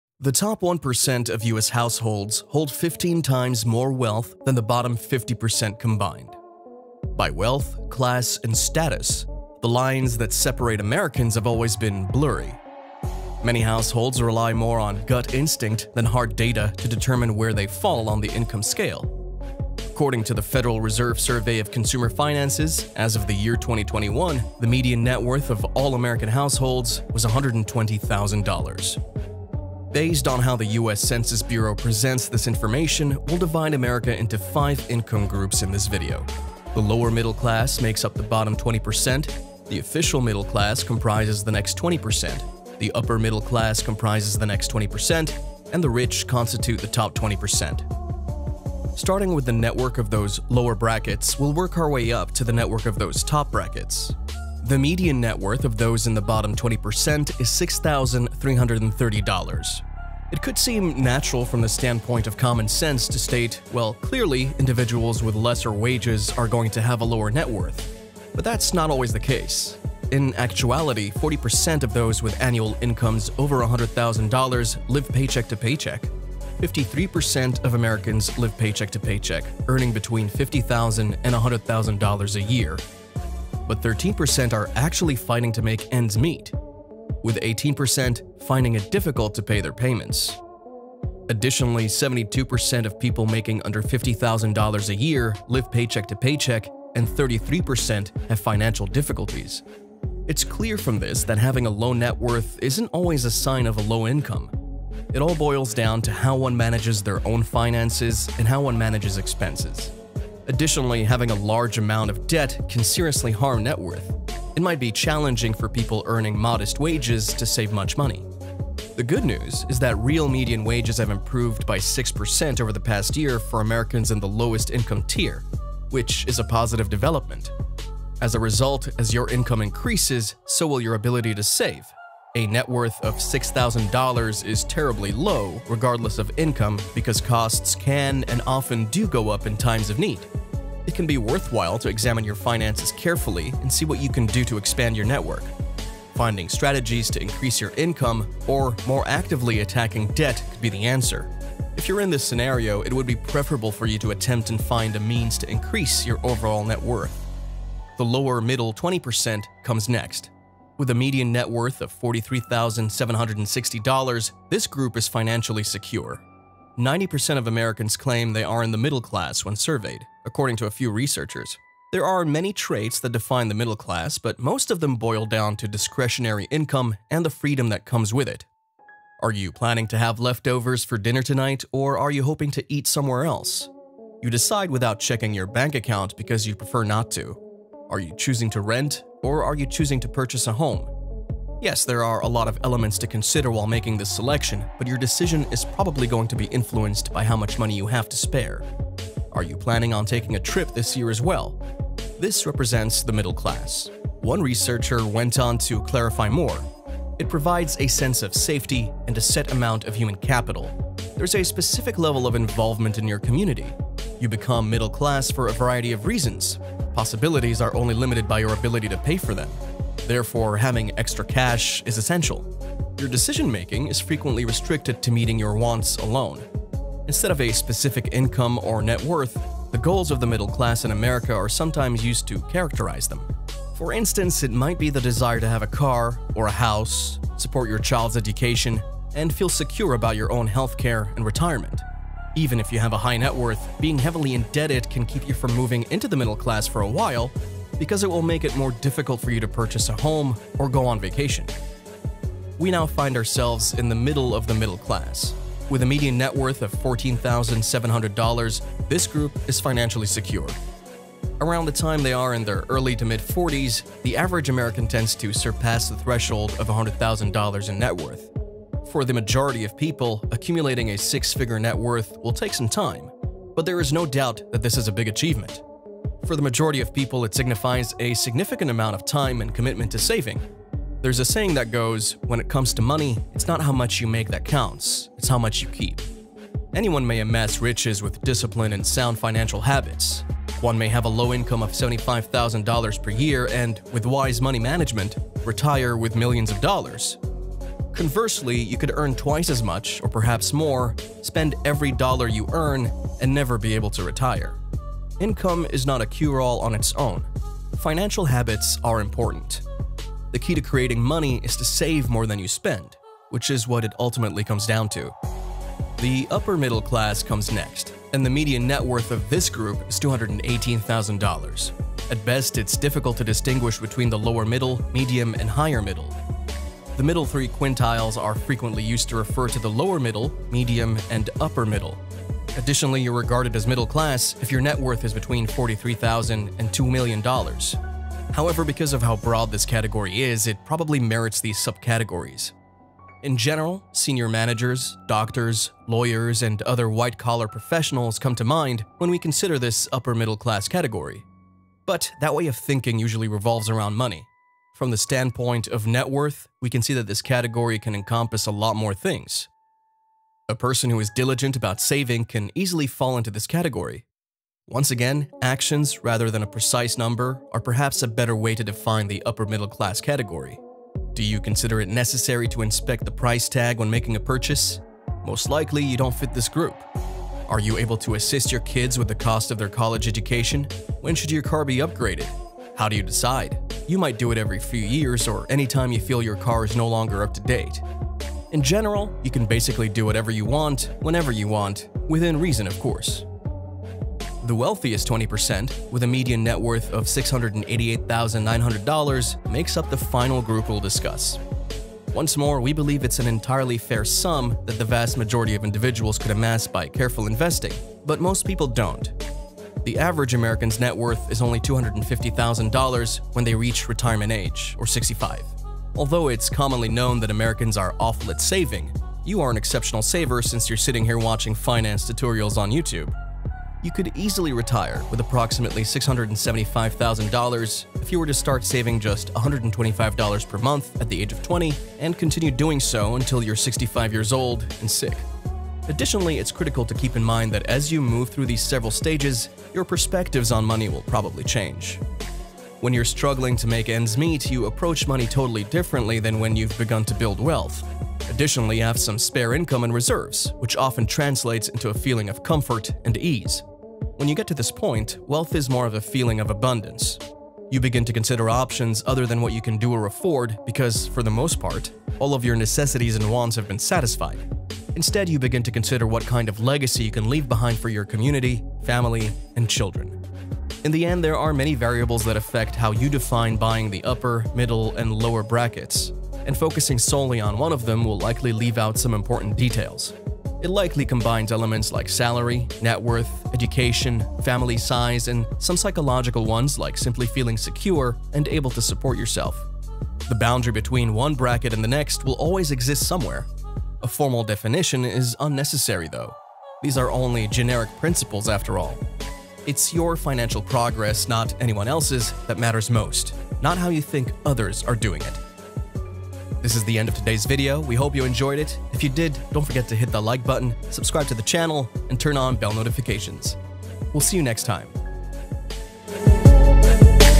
How Much Net Worth Is Needed To Be in America's Upper, Middle & Lower Social Class? | US Economy
By Net Worth, class, and status, the lines that separate Americans have always been blurry. Many households rely more on gut instinct than hard data to determine where they fall on the Net Worth scale. According to the Federal Reserve Survey of Consumer Finances, as of the year 21, the median net worth of all American households was $120,000. Based on how the U.S. Census Bureau presents this information, we will divide America into five income groups in this video. The lower middle class makes up the bottom 20%, the official middle class comprises the next 20%, the upper middle class comprises the next 20%, and the rich constitute the top 20%.

How Much In Net Worth Are Needed To Be in America's Upper, Middle & Lower Class?

Timestamps:
0:00 Intro
0:48 Starting with the network of those lower brackets
2:38 The Lower Middle 20% Comes Next
4:12 We now find ourselves in the middle of the middle class
5:18 The Upper Middle Class comes next
6:34 The Wealthiest 20%

*You can learn the secret of growing your trading account:*